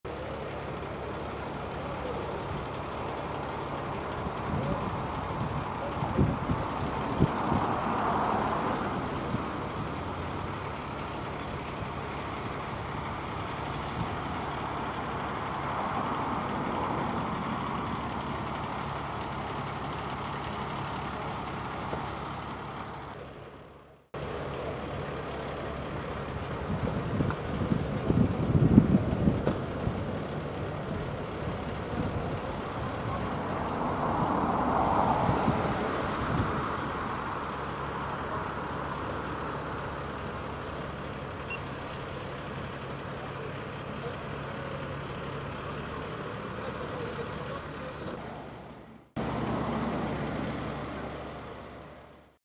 Brandweer en Ambulance met spoed naar een ernstig ongeval in Rotterdam - Ijsselmonde!
Brandweer Officier van Dienst en Ambulance Rapid Repsponder met spoed naar een ernstig ongeval in Rotterdam - Ijsselmonde!